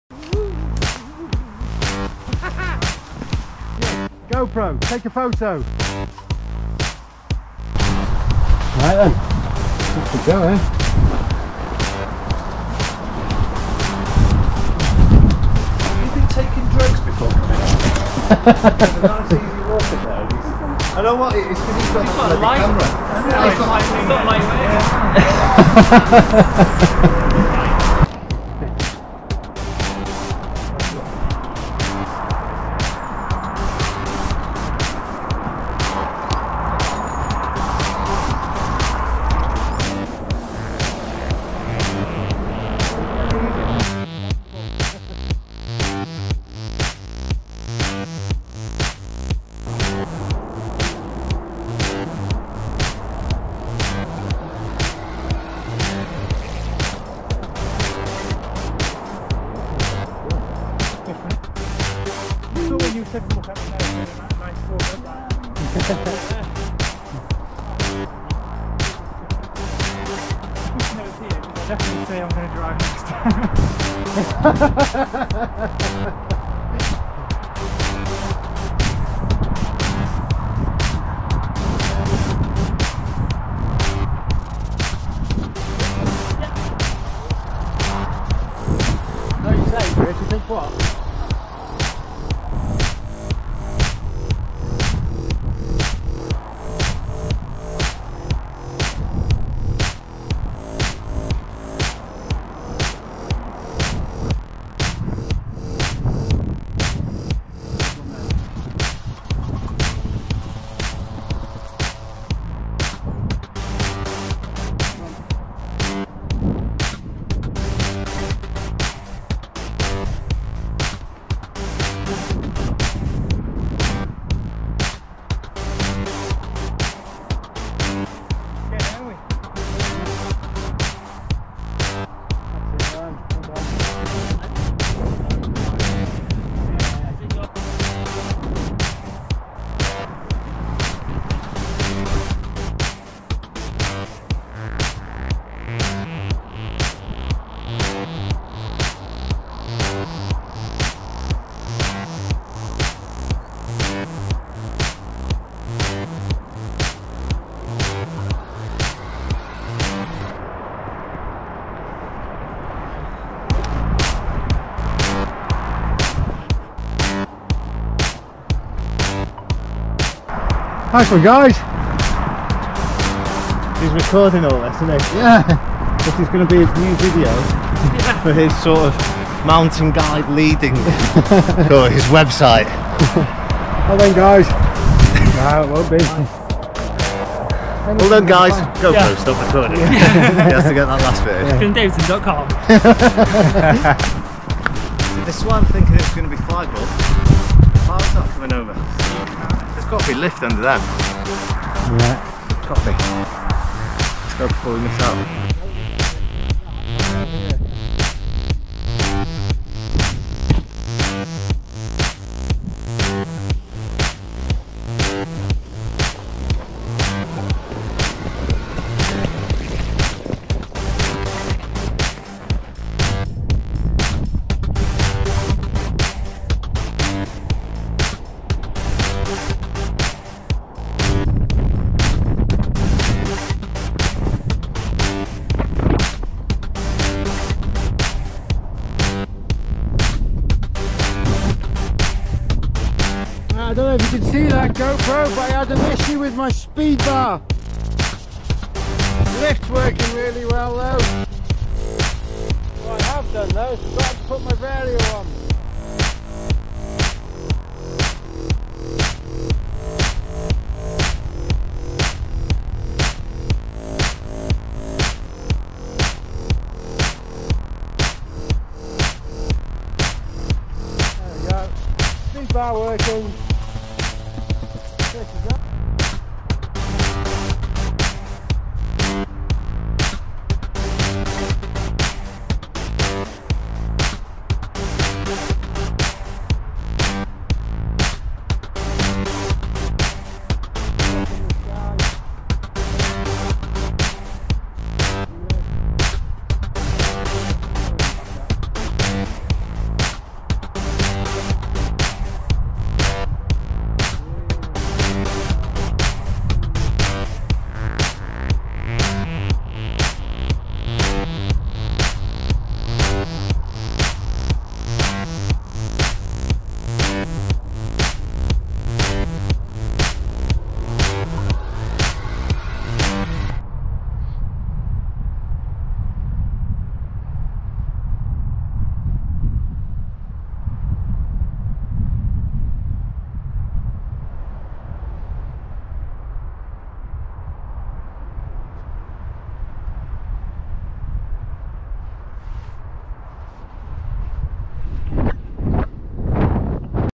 Paragliding at Penmaenbach, Conwy
Trying out the GoPro Max for the first time. I must say that I'm really impressed with it. Such a useful feature being able to decide where to point the camera in post.